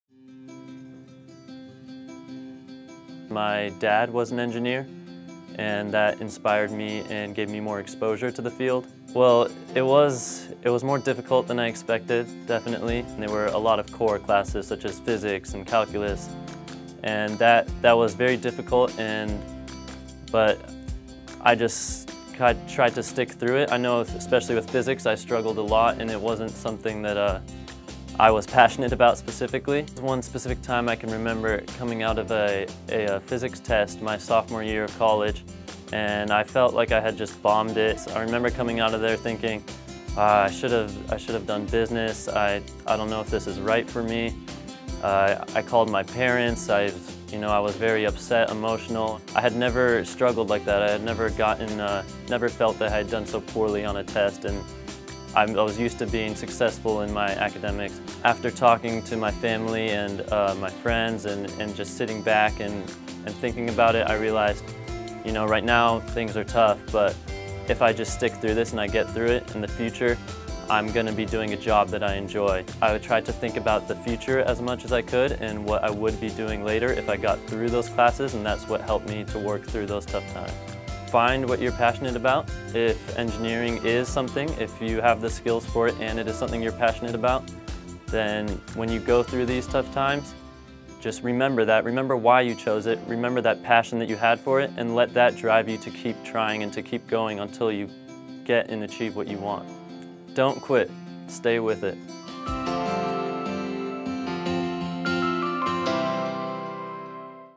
Industrial Engineering Student Brandon Parsiani Says Stay With It
Meet Brandon.  Brandon Parsiani is currently a student working on his Bachelor’s degree in Industrial Engineering at Oregon State University.  Brandon was inspired to become engineer by his father who also is an engineer.  Though he had a lot of exposure to engineering, it was still more difficult that he thought it would be.  He struggled a lot with Physics; after his first exam, he was upset and emotional and felt like he completely failed.  Brandon even thought about changing his major to business because he has never done so poorly in school.   He spoke with his parents and friends and they encouraged him not to give up and stick with it. Brandon, decided to shift his thinking and look towards his future and his passion.   Brandon’s advice for students who may be thinking about quitting or changing their majors due to challenges with courses,  “Find what you are passionate about…when you go through tough times, just remember that passion and let that drive you to keep going until you achieve what you want!”